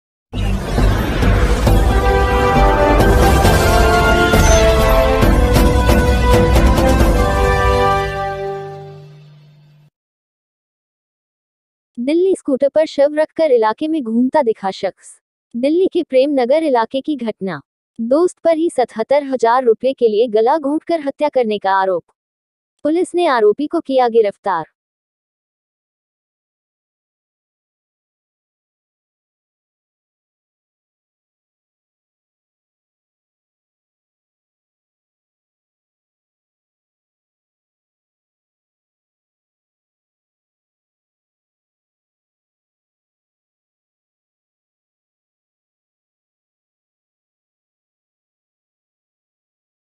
दिल्ली-स्कूटर पर शव रखकर इलाके में घूमता दिखा शख्स, दिल्ली के प्रेम नगर इलाके की घटना.....
दिल्ली: स्कूटर पर शव रखकर इलाके में घूमता दिखा शख्स, दिल्ली के प्रेम नगर इलाके की घटना। दोस्त पर ही 77 हजार रुपये के लिए गला घोंटकर हत्या करने का आरोप, पुलिस ने आरोपी को किया गिरफ्तार।